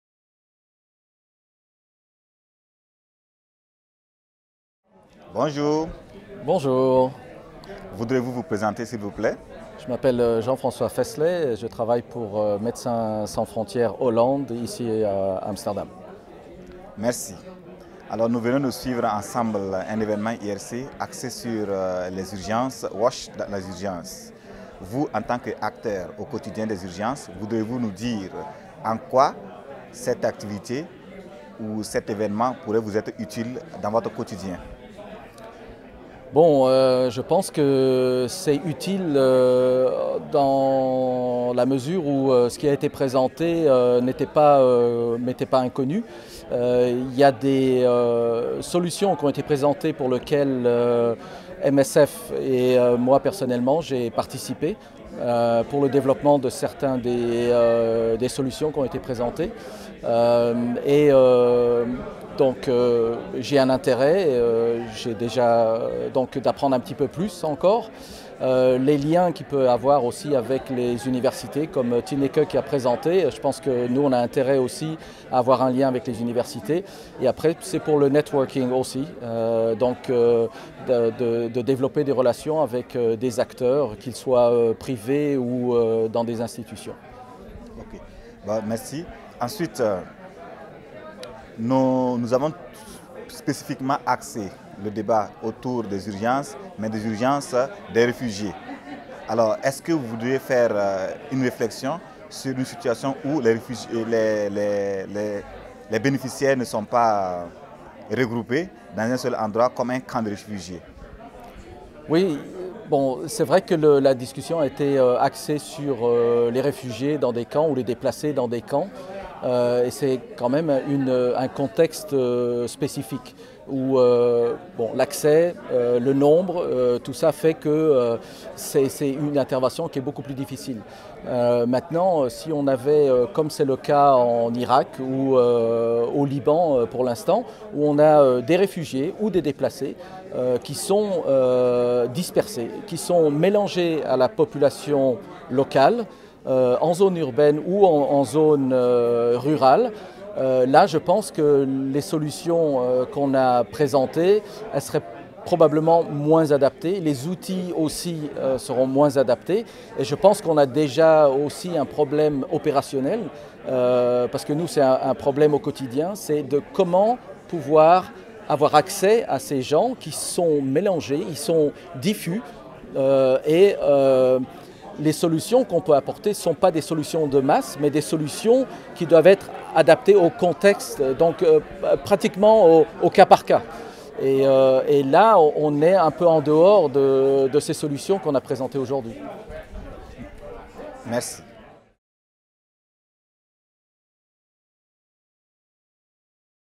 IRC Interview en Francais, Celestin Badolo, Innovative approaches to WASH in emergency situations
Interview Celestin Badolo, IRC Burkina avec Jean-Francois Fesselet, MSF